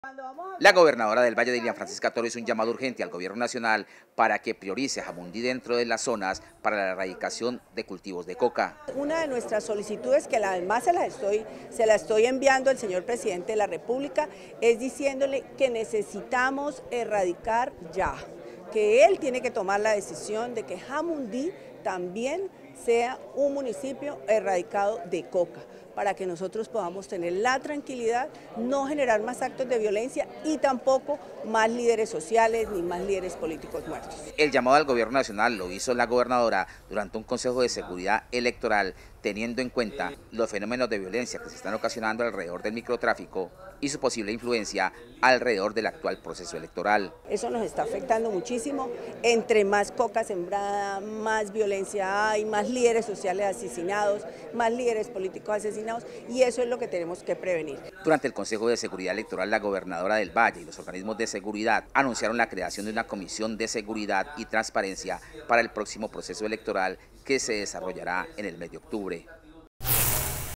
Gobernadora reclamó al gobierno nacional priorizar la erradicación de cultivos de coca en Jamundí
Emitido el 2 de Julio por el Canal regional Telepacífico.